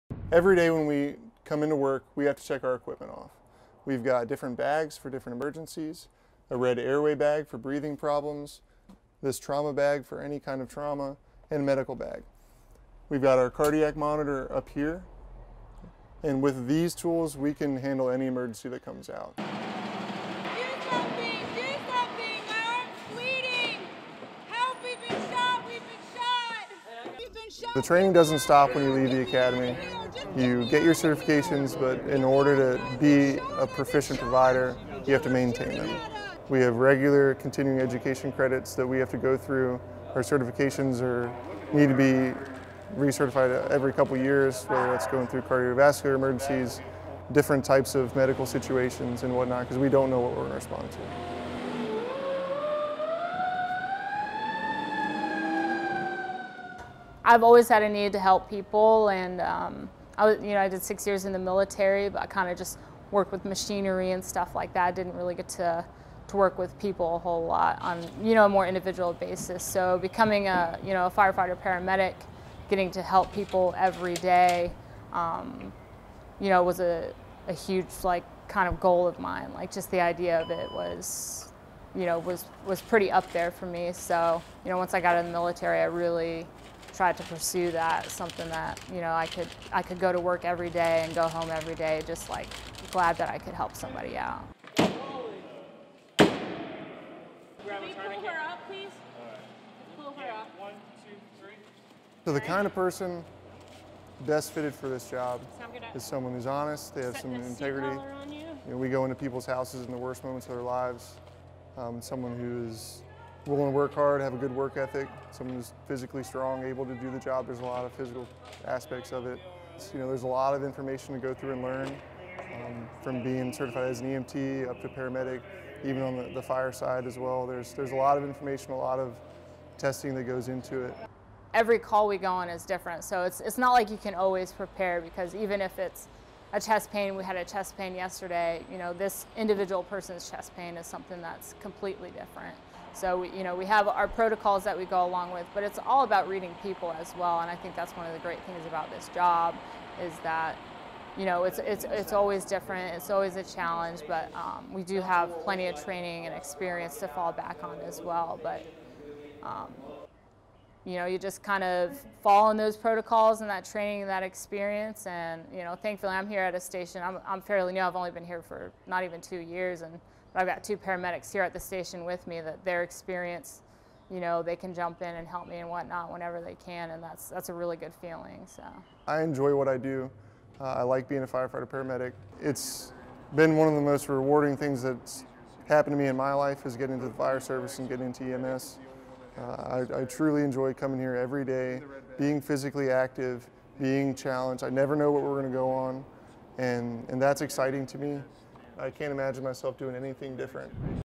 Chesapeake Weekly, National EMS Week
It’s National EMS Week! We’re proud to take a moment to thank the men and women who literally come to our rescue when we need them most. EMS workers in Chesapeake are part of the Fire Department and are trained in a variety of rescue skills.